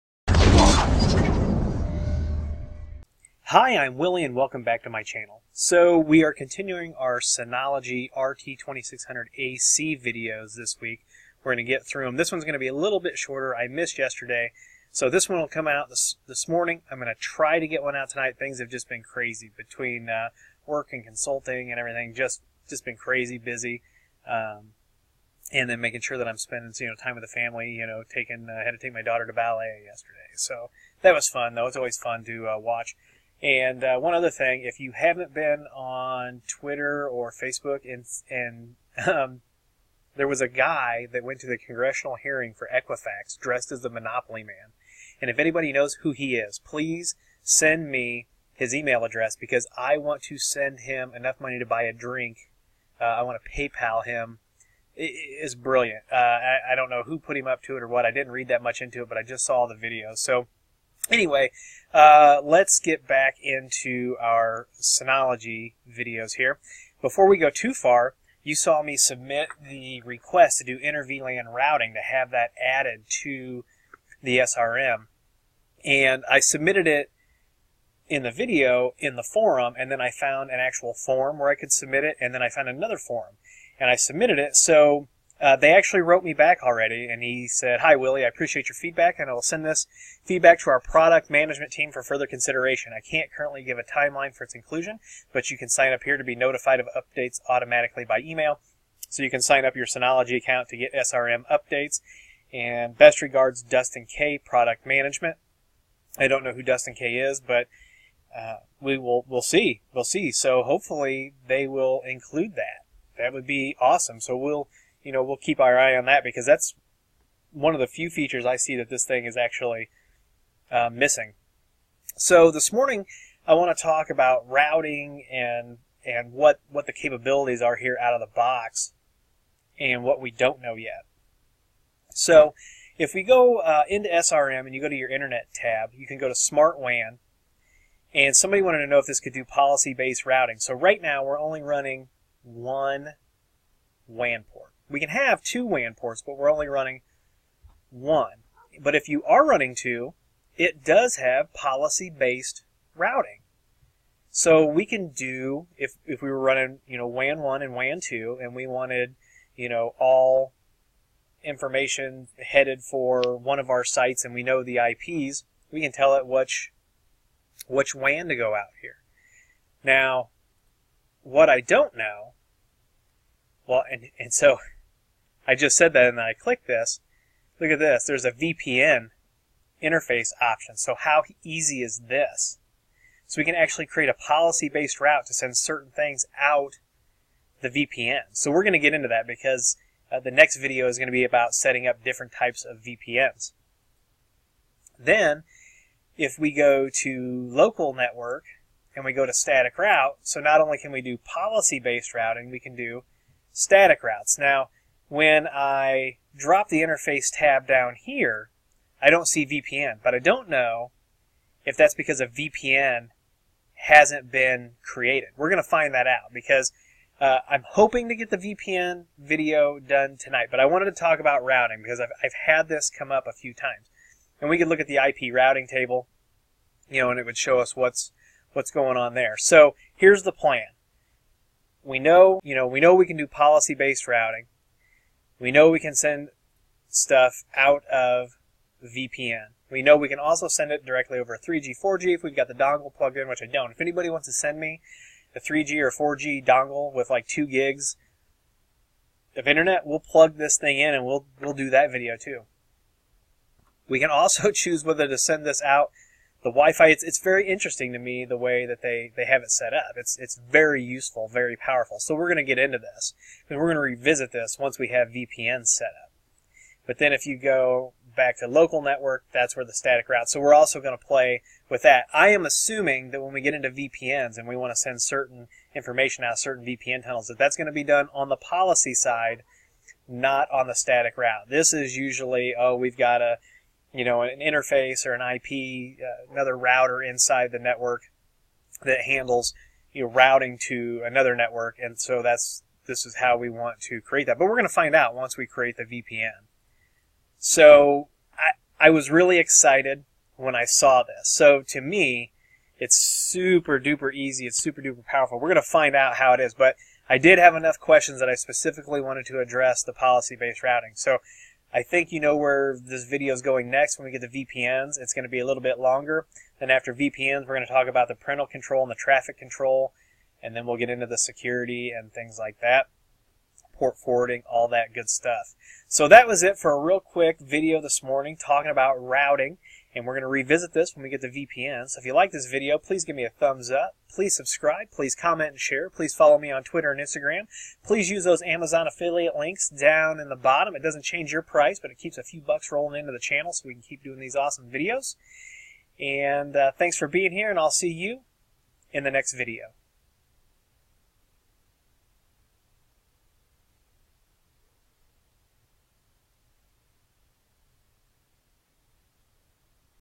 Synology RT2600AC Route Options
Thanks Mike!  Mike sent me a Synology RT2600AC.  I have had several questions about the ability of this router to do static VLANs and PBR (policy based routing).  It can do both static routes and policy based routing.  We'll expand on both of these in the next video!  Synology has already responded to our request to be able to add inter-vlan routing.  Hopefully it gets included!!

Synology RT2600AC
UniFi AC Pro